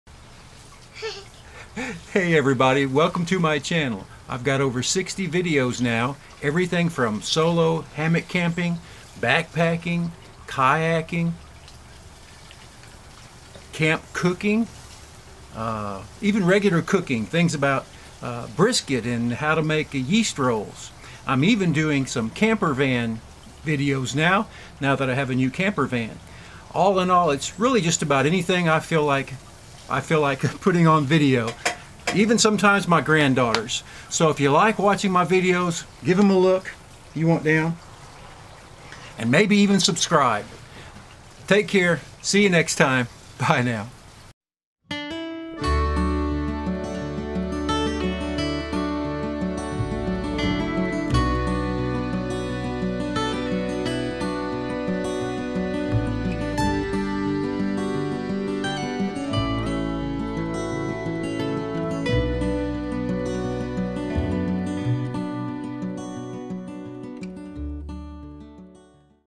Channel Trailer
I'm Ken Click and my videos are mostly about cooking, but also solo hammock camping, kayaking, camper-van camping, and a few other things. Hope you like them!